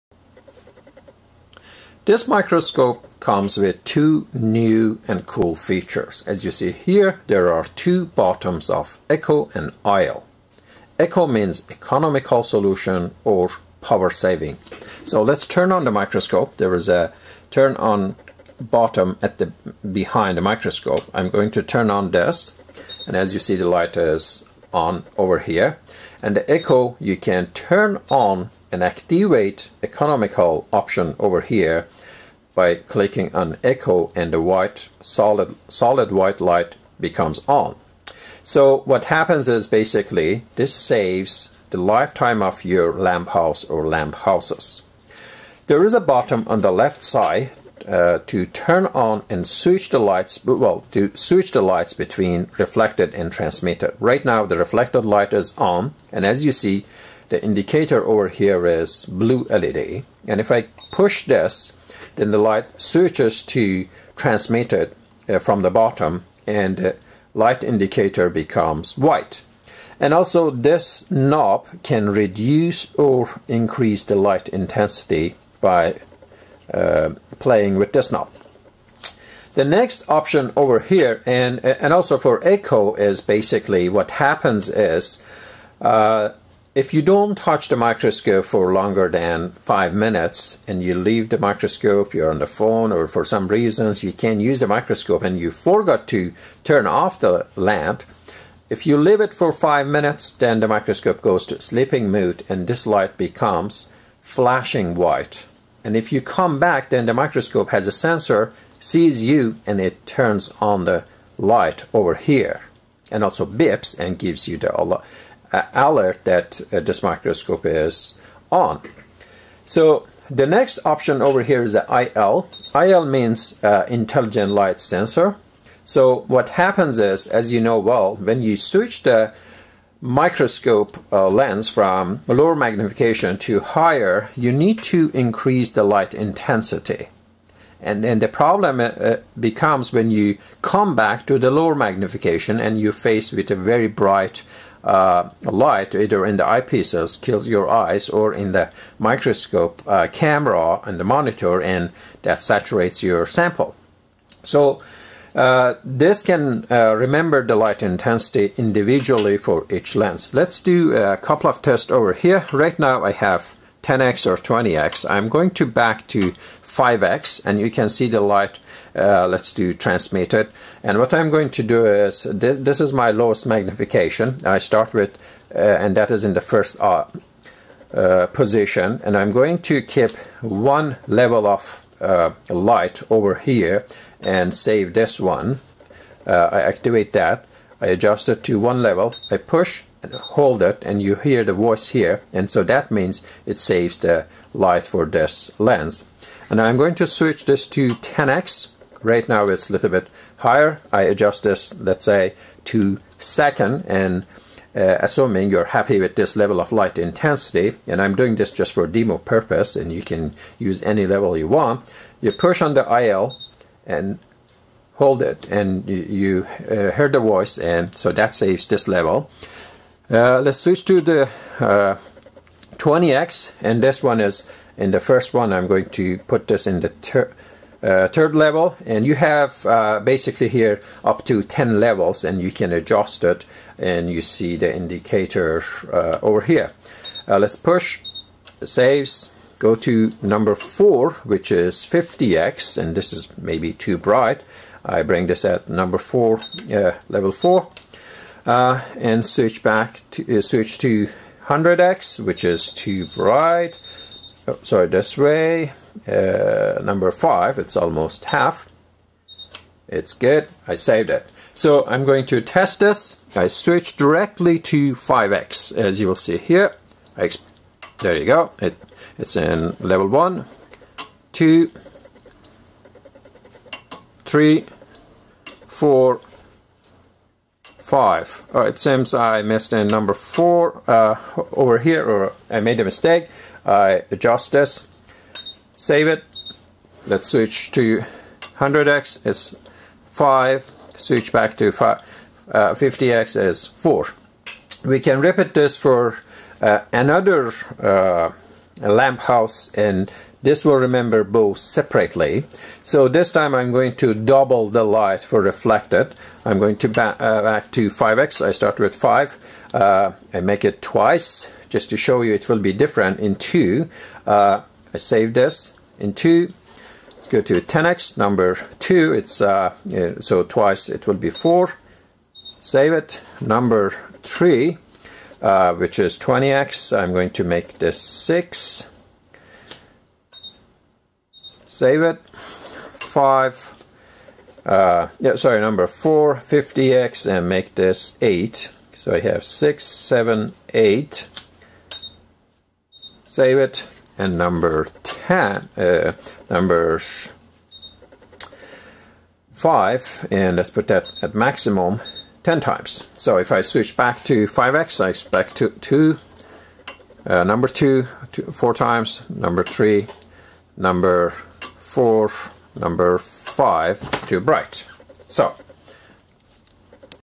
Bioimager Video Tutorial on Huvitz HRM300 Upright Metallurgical 3D Profiler- Part 2: Illumination
Bioimager Video Tutorial on Huvitz HRM300 Upright Metallurgical Microscope with 3D Profiler, bright field, darkfield, polarized & Nomarski DIC imaging